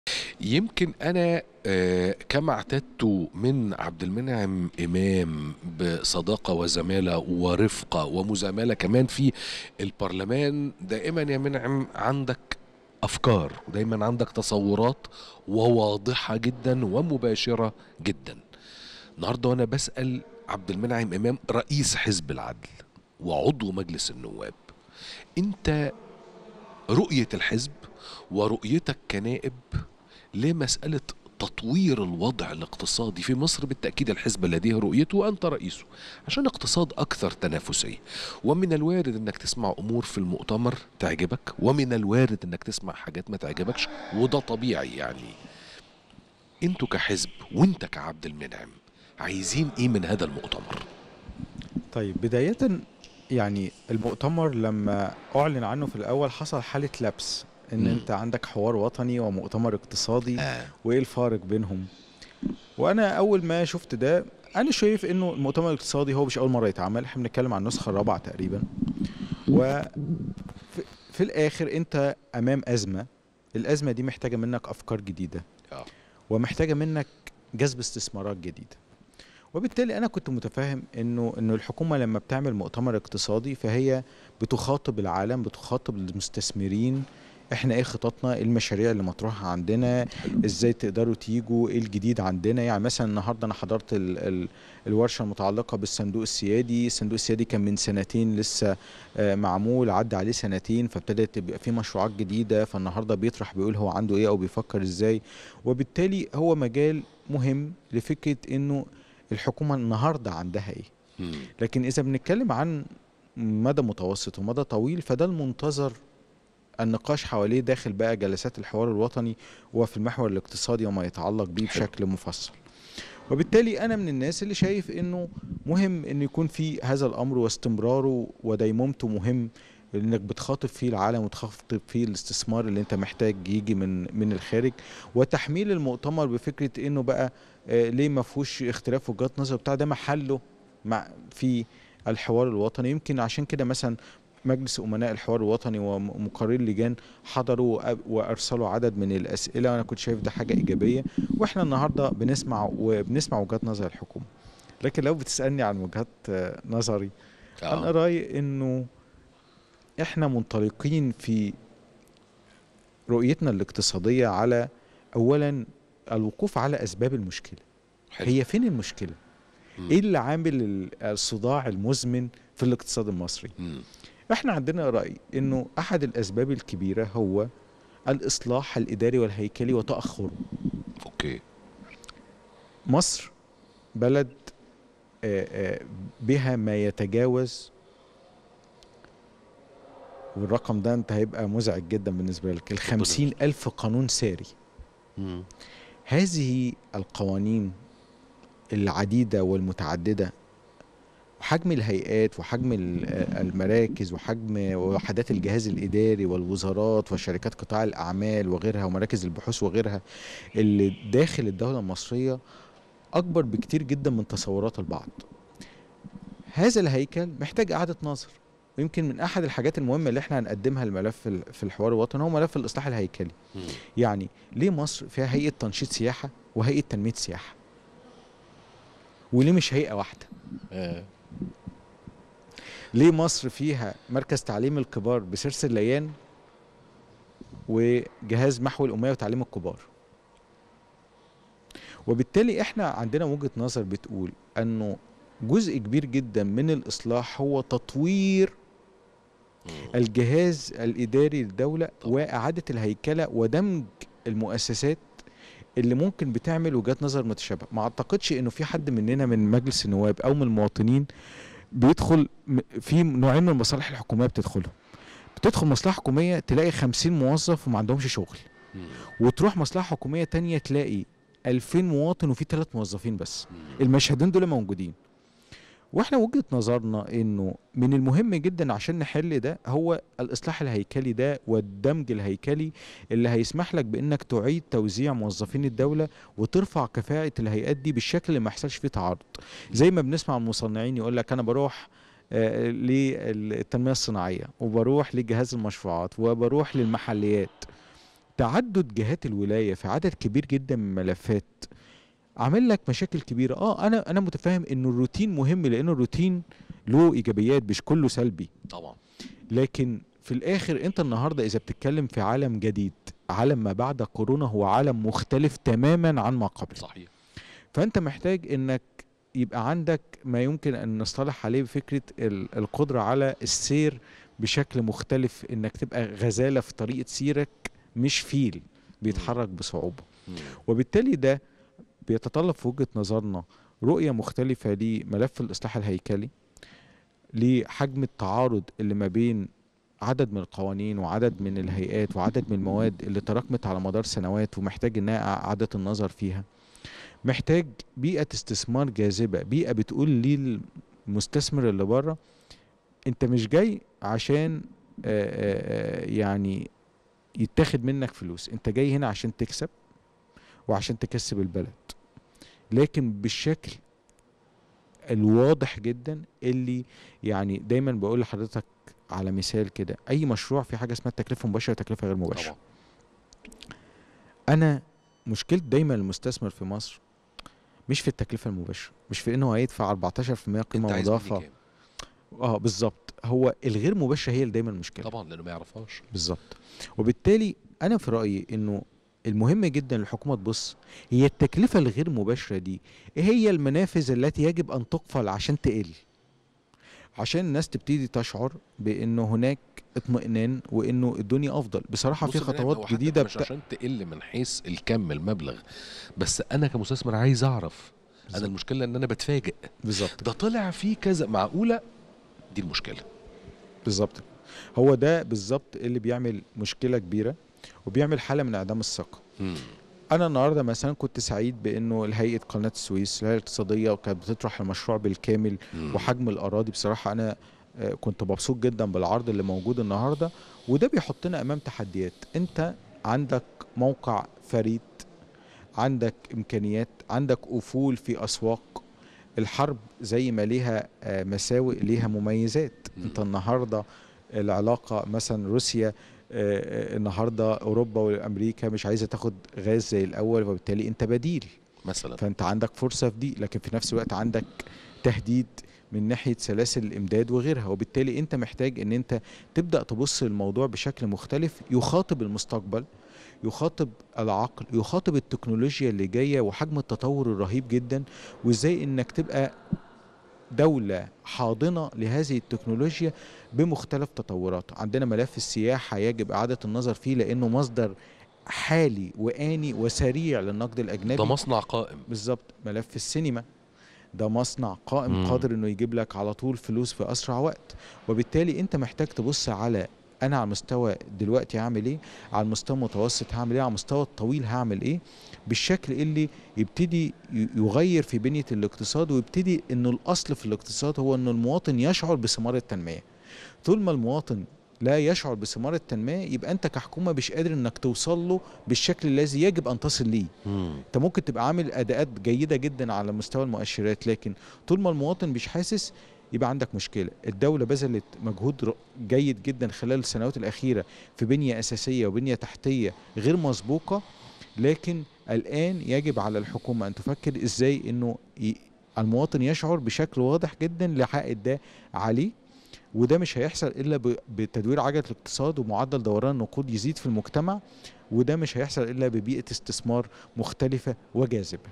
عبد المنعم إمام "رئيس حزب العدل وعضو مجلس النواب"يوضح رؤيته ورؤية الحزب في المؤتمر الاقتصادي مصر2022
التاسعة | عبد المنعم إمام " رئيس حزب العدل وعضو مجلس النواب" يوضح رؤيته ورؤية الحزب في المؤتمر الاقتصادي " مصر2022"